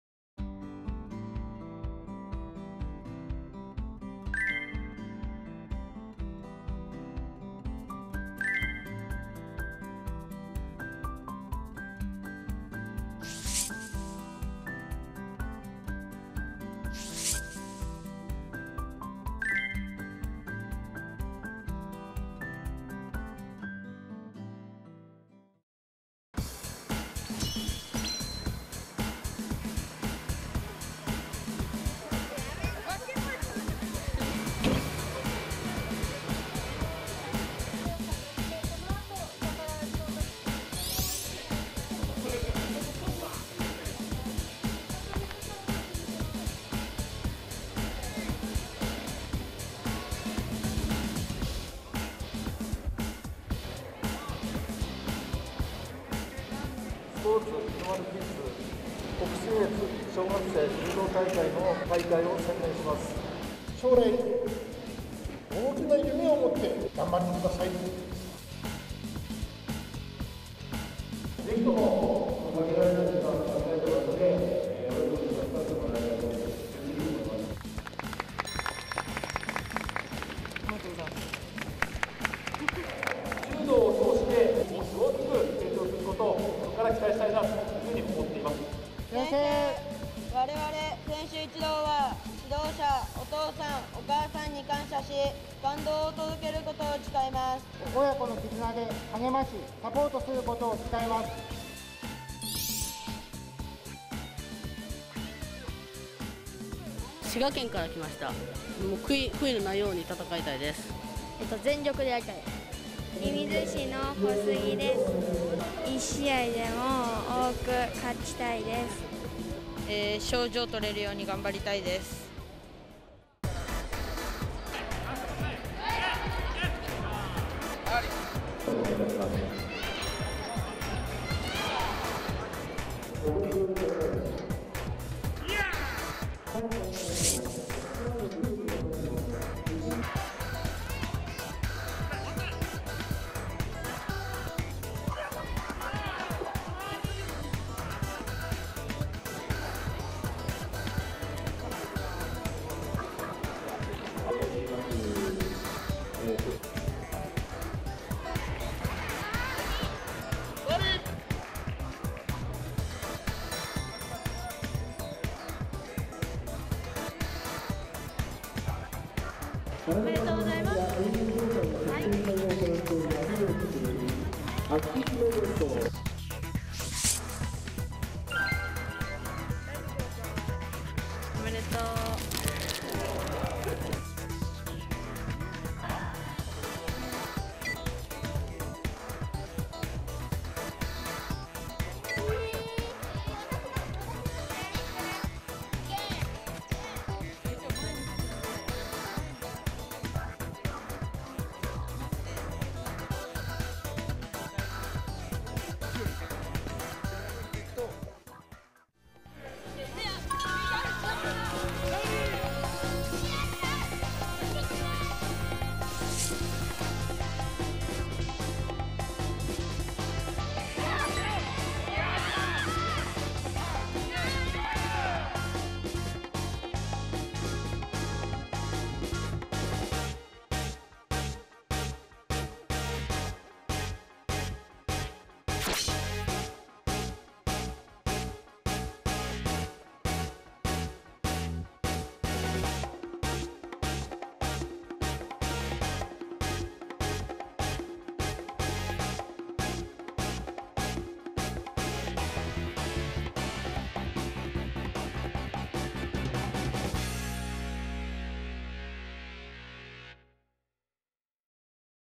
第12回スポーツひのまるキッズ北信越小学生柔道大会ダイジェスト
2021年10月31日(日)に富山県射水市のアルビス小杉総合体育センターにて
第12回スポーツひのまるキッズ北信越小学生柔道大会を行ないました。
新型コロナウイルス感染症の感染対策を講じながら、
大会を開催いたしました。
その様子をまとめておりますので、ぜひご覧ください。

ひのまるキッズ情報

★スタッフによる親子の絆ブログはこちら